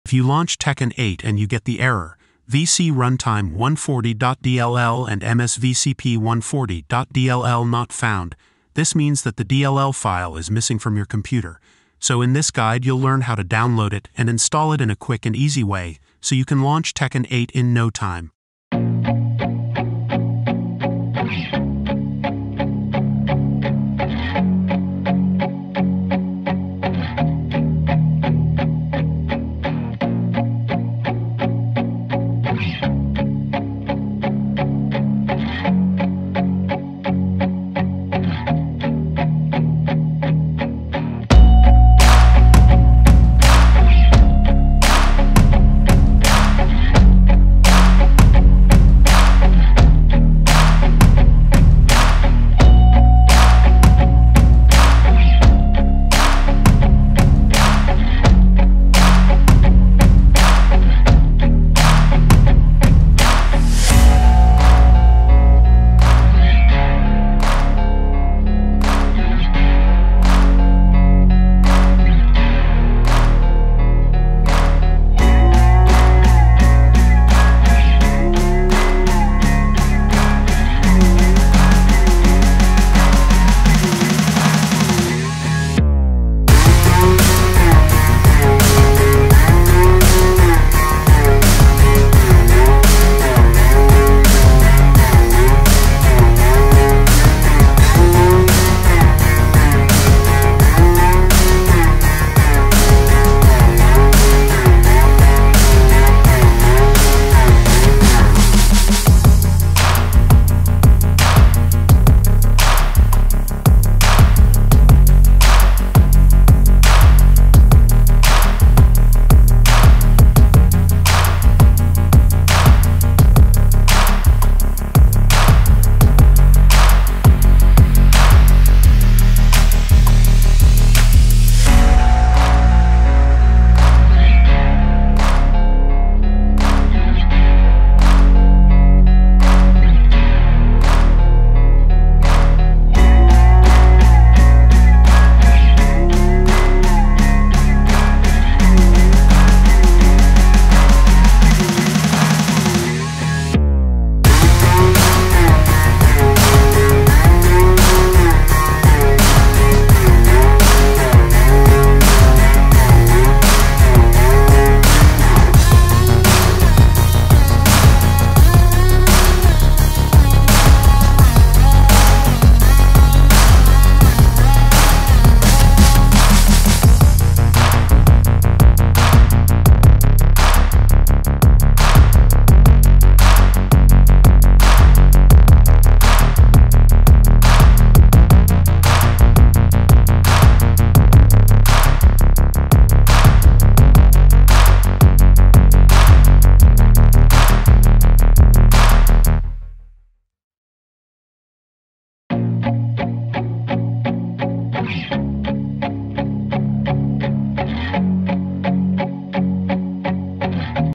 Tekken 8 Microsoft Visual C++ Runtime Error VCRUNTIME140.dll and MSVCP140.dll FIX
If you launch Tekken 8 and you get the error: VCRUNTIME140.dll and MSVCP140.dll not found, this means that the dll file is missing from your computer, so in this tutorial we'll teach you how to download it and install it in a quick and easy way so you can launch  Tekken 8 in no time.

In this video I am going to show How to FIX Tekken 8 Missing VCRUNTIME140.dll and MSVCP140.dll Files

00:00 Intro
00:20 Solution #1
03:25 Outro